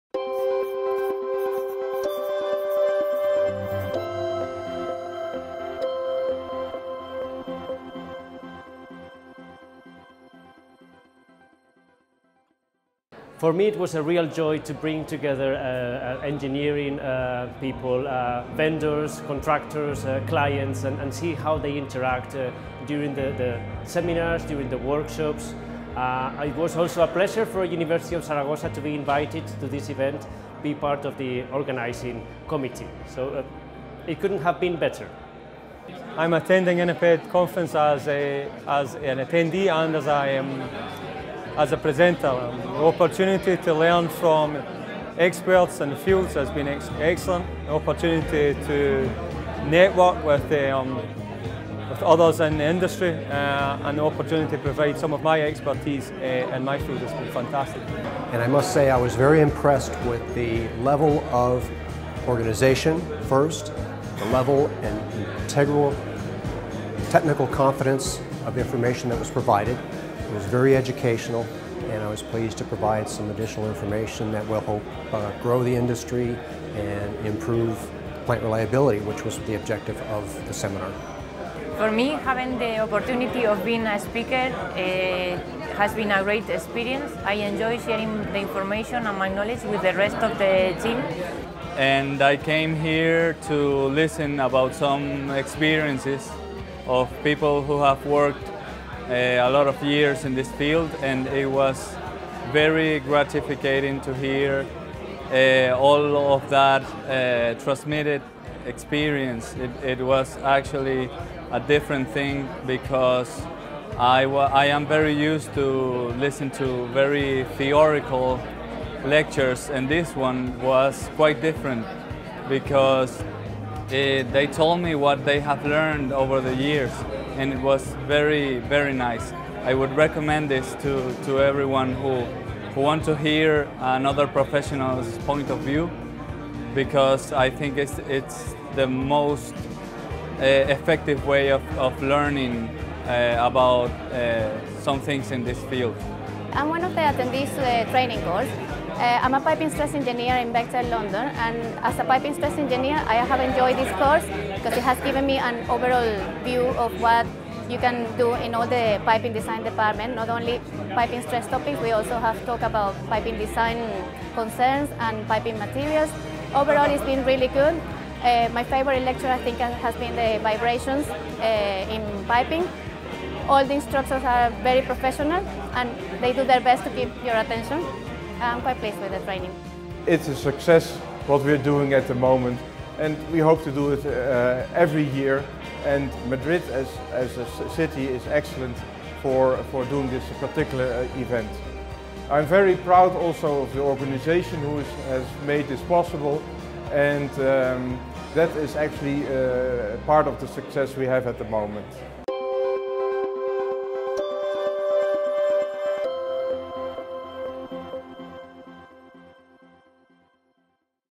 PEW2016 - Plant Engineering Workshop 2016
Briefing from Plant Engineering Workshop 2016. Celebrated on Madrid during October 22nd and 23rd 2016. We brought the opportunity to share knowledge and learn from experts of different fields of plant engineering. The opportunity to meet and network with other proffesionals from the industry and bring together new commercial relations.